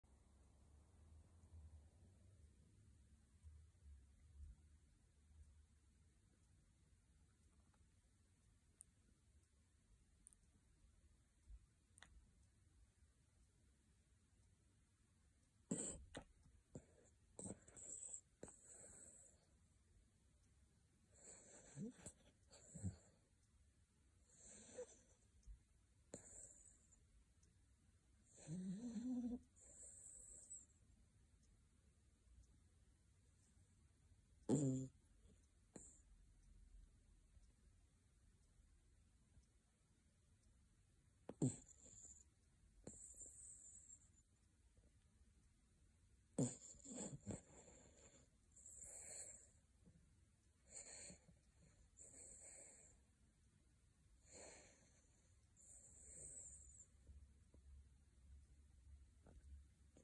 Kleiner Havaneser Bert schnarcht 😂❤️ (laut hören!)
Die Lautstärke muss hoch eingestellt sein. 👍